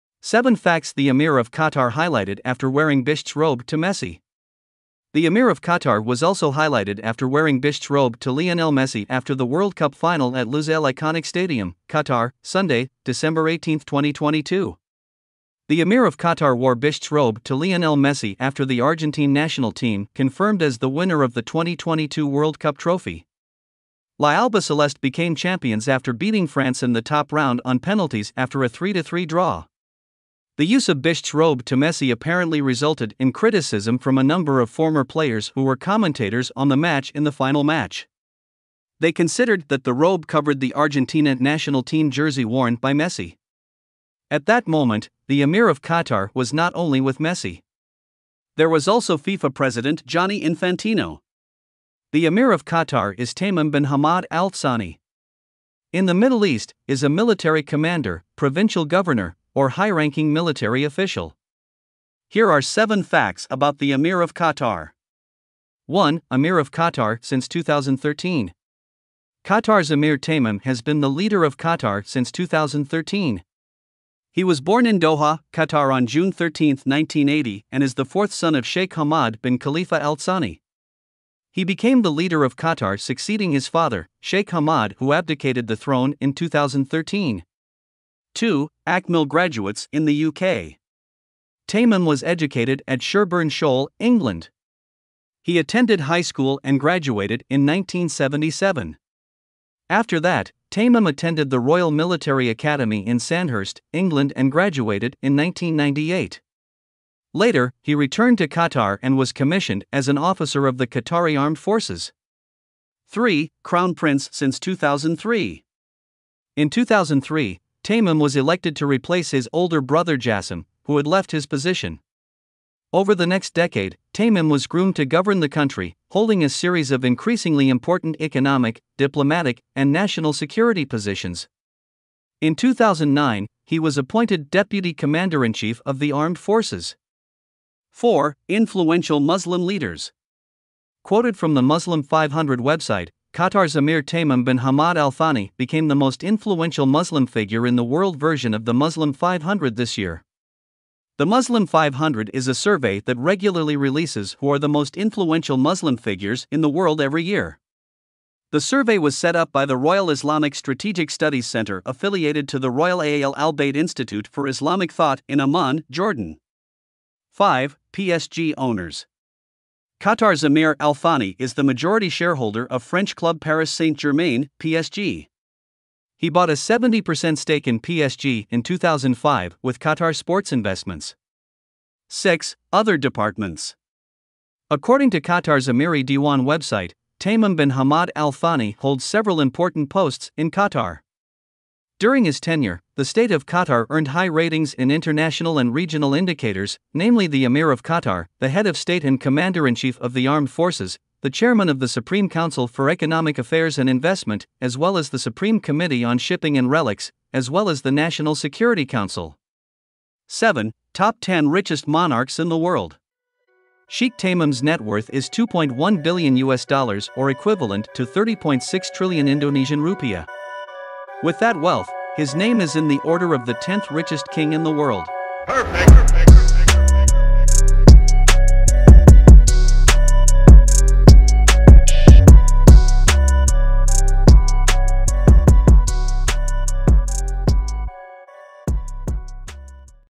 7 Facts The Emir of Qatar highlighted after wearing Bisht's robe to Messi
7 Facts The Emir of Qatar highlighted after wearing Bisht's robe to Messi

The Emir of Qatar was also highlighted after wearing bisht's robe to Lionel Messi after the World Cup final at Lusail Iconic Stadium, Qatar, Sunday (18/12/2022).
The Emir of Qatar wore bisht's robe to Lionel Messi after the Argentine national team confirmed as the winner of the 2022 World Cup trophy. La Albiceleste became champions after beating France in the top round on penalties after a 3-3 draw.

The use of bisht's robe to Messi apparently resulted in criticism from a number of former players who were commentators on the match in the final match. They considered that the robe covered the Argentina national team jersey worn by Messi.

At that moment, the Emir of Qatar was not only with Messi. There was also FIFA President Gianni Infantino.

The Emir of Qatar is Tamim bin Hamad Al Tsani. In the Middle East, is a military commander, provincial governor, or high-ranking military official.

Here are 7 facts about the Emir of Qatar:
1. Emir Of Qatar Since 2013
Qatar's Emir Tamim has been the leader of Qatar since 2013. He was born in Doha, Qatar on 13 June 1980 and is the fourth son of Sheikh Hamad bin Khalifa Al-Tsani.

He became the leader of Qatar succeeding his father, Sheikh Hamad who abdicated the throne in 2013.

2. Akmil graduates in the UK
Tamim was educated at Sherborne Schhol, England. He attended high school and graduated in 1977. After that, Tamim attended the Royal Military Academy in Sandhurst, England and graduated in 1998. Later, he returned to Qatar and was commissioned as an officer of the Qatari Armed Forces.

3. Crown Prince Since 2003
In 2003, Tamim was elected to replace his older brother Jassim, who had left his position.

Over the next decade, Tamim was groomed to govern the country, holding a series of increasingly important economic, diplomatic, and national security positions. In 2009, he was appointed deputy commander-in-chief of the Armed Forces.

4. Influential Muslim Leaders
Quoted from the Muslim 500 website, Qatar's Emir Tamim bin Hamad Al-Thani became the most influential Muslim figure in the world version of the Muslim 500 this year. The Muslim 500 is a survey that regularly releases who are the most influential Muslim figures in the world every year.

5. PSG owners
Qatar's Emir Al Thani is the majority shareholder of French club Paris Saint Germain (PSG). He bought a 70 percent stake in PSG in 2005 with Qatar Sports Investments.

6. Other Departments
According to Qatar's Amiri Diwan website, Tamim bin Hamad Al Thani holds several important posts in Qatar. During his tenure, the state of Qatar earned high ratings in international and regional indicators, namely the emir of Qatar, the head of state and commander-in-chief of the Armed Forces, the chairman of the Supreme Council for Economic Affairs and investment, as well as the Supreme Committee on shipping and relics, as well as the National Security Council.

7. Top 10 richest monarchs in the world
Sheikh Tamim's net worth is USD 2.1 billion or equivalent to Rp 30.6 trillion. With that wealth, his name is in the order of the tenth richest king in the world.